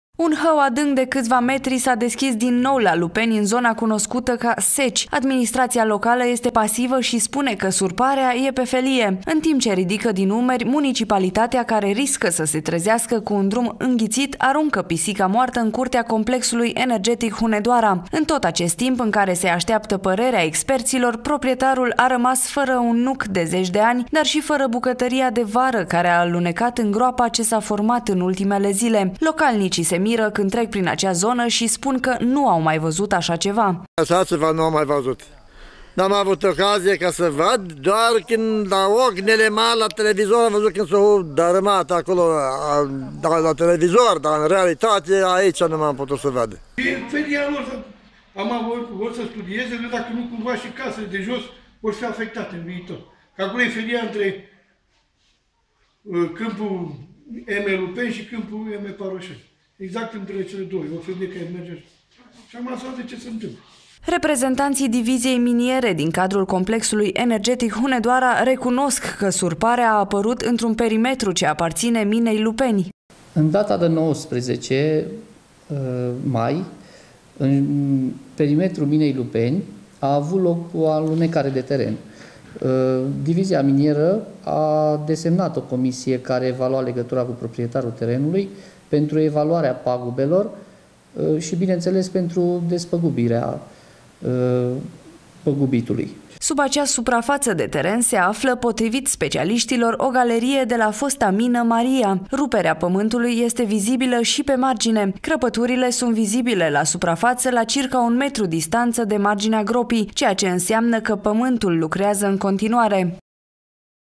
SURPARE DE TEREN LA LUPENI
Catastrofa naturală s-a produs pe strada Victoria din localitate